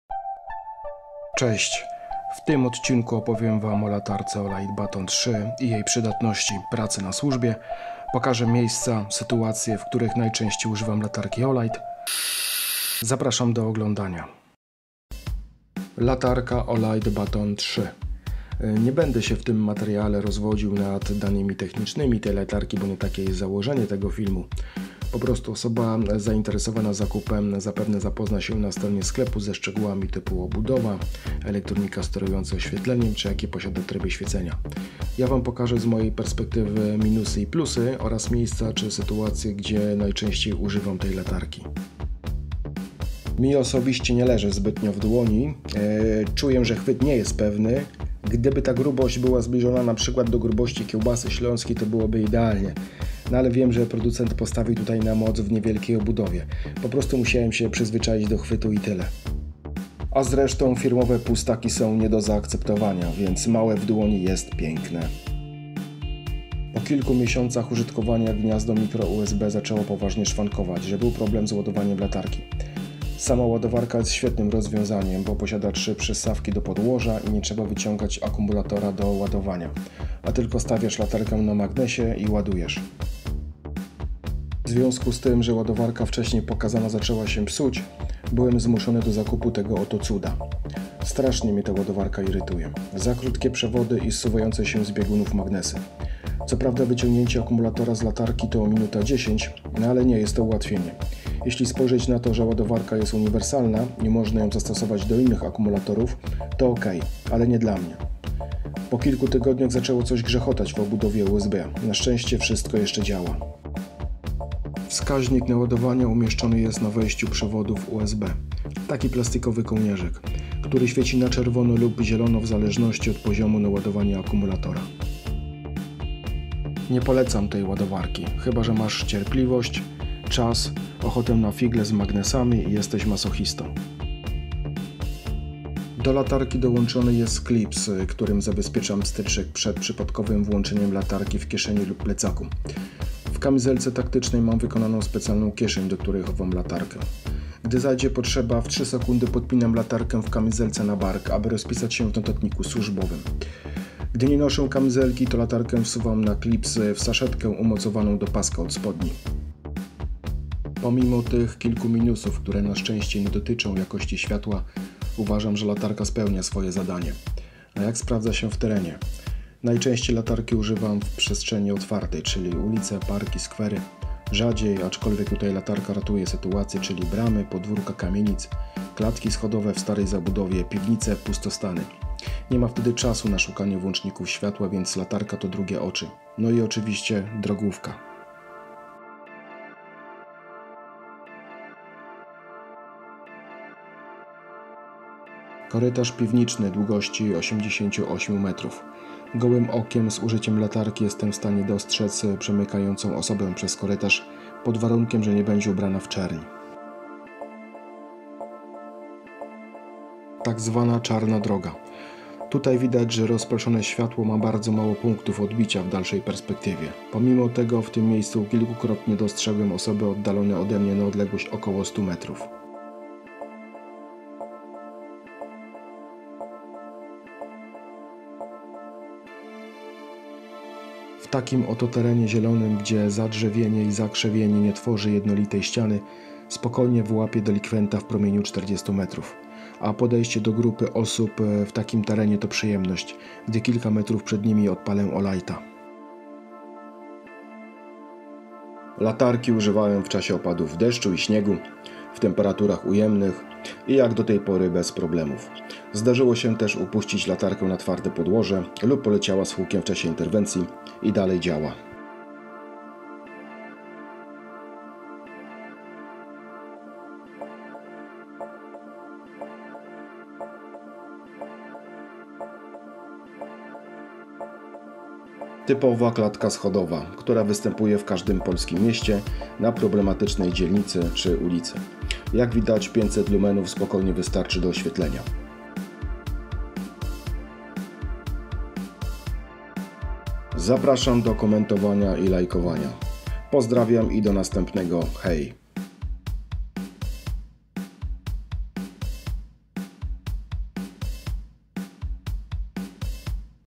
Latarka patrolowa - mój wybór, test, opinia, Olight Baton
Krótkie omówienie latarki, którą zakupiłem w 2018 roku z myślą o pewnym i niezawodnym wyposażeniu w służbie. W materiale nie poruszyłem tematu związanego z zasadami bezpieczeństwa podczas czynności legitymowania, kontroli osobistej i drogowej z użyciem latarki. Są to zagadnienia na znacznie dłuższy odcinek.
Jeśli zastanawiasz się nad zakupem latarki do patrolowania to być może mój film ułatwi twój wybór.